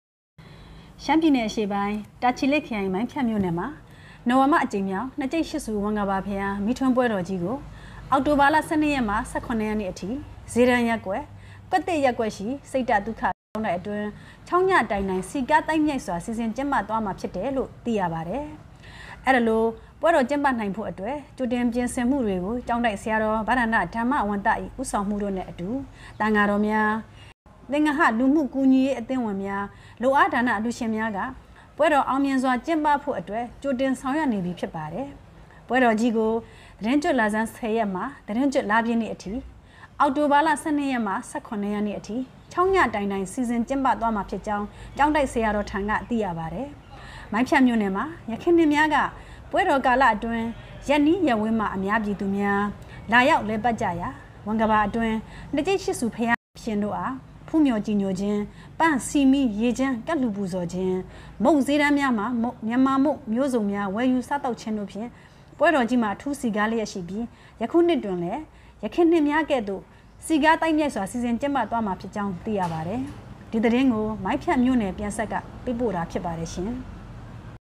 နှစ်ကျိပ်ရှစ်ဆူဝင်္ကပါဘုရားမီးထွန်းပွဲတော် ခြောက်ညတိုင်တိုင် စည်ကားသိုက်မြိုက်စွာ ကျင်းပမည်
မိုင်းဖြတ်မြို့နယ်၌ နှစ်ကျိပ်ရှစ်ဆူဝင်္ကပါဘုရားမီးထွန်းပွဲတော် ခြောက်ညတိုင်တိုင် စည်ကားသိုက်မြိုက်စွာ ကျင်းပမည်
မိုင်းဖြတ် အောက်တိုဘာ ၈
သန်းထွန်းအေး